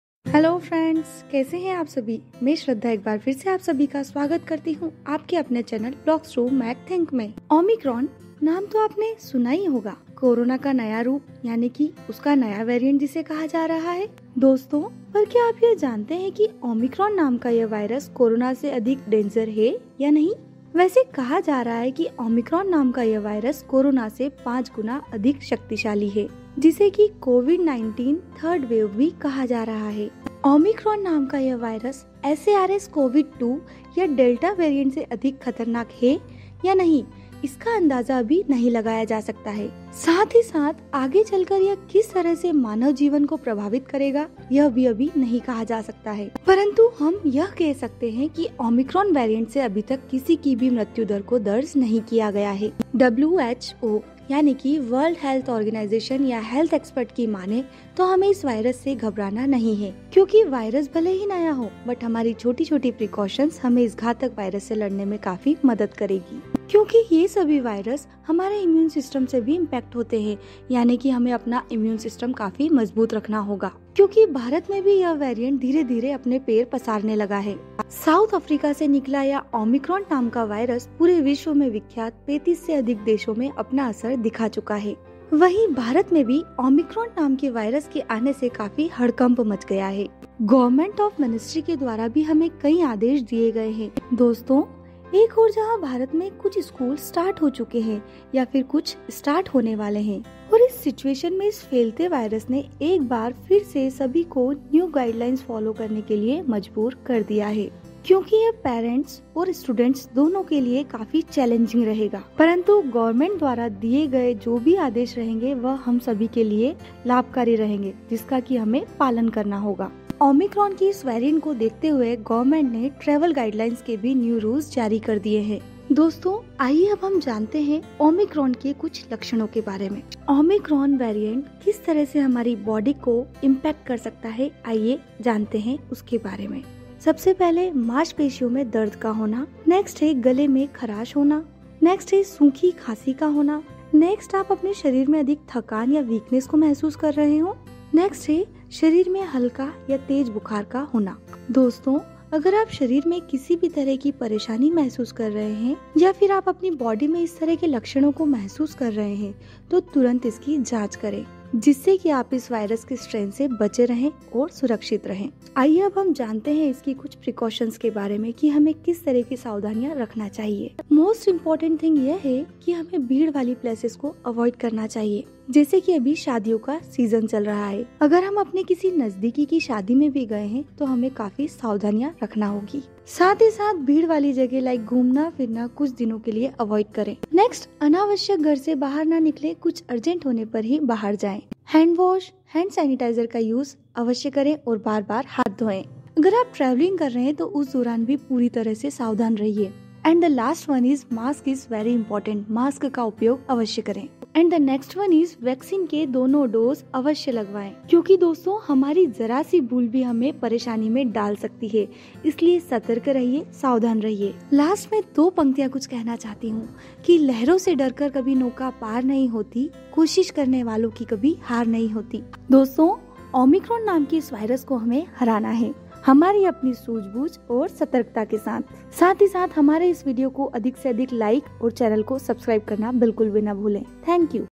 Omicron Virus | New Variant Of CoVid-19 | SARS-CoV-2 virus | Impact | Symptoms | Precautions
दक्षिण अफ्रीका से आया Corona का नया Variant Omicron अब भारत में भी अपने पैर पसारने लगा है। तो आइए जानते हैं इसके लक्षण, प्रभाव एवं सावधानियों के बारे में।

Immune System मजबूत करिए और कोरोना से दूर रहे | Strong Immunity System Keeps Corona Virus Away